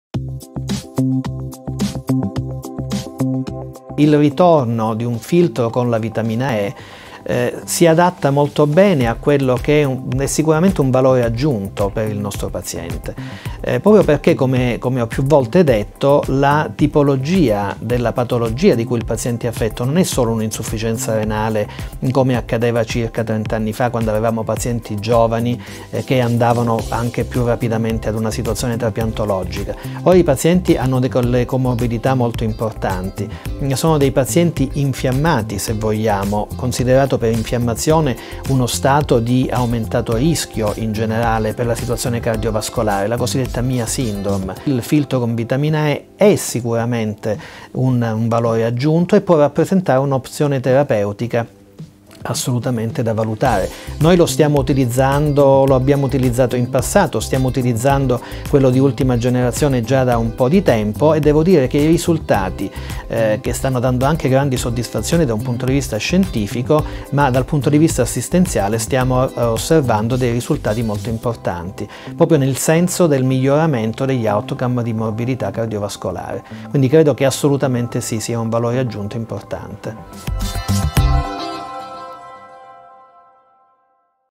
La vitamina E in dialisi - Dott. Paolo Monardo
Quale valore può portare il filtro con vitamina E nel trattamento dialitico? L’abbiamo chiesto al dott. Paolo Monardo, Direttore UOC di Nefrologia e Dialisi dell’Ospedale Papardo di Messina.

Scopri di più sui filtri con vitamina E per la dialisi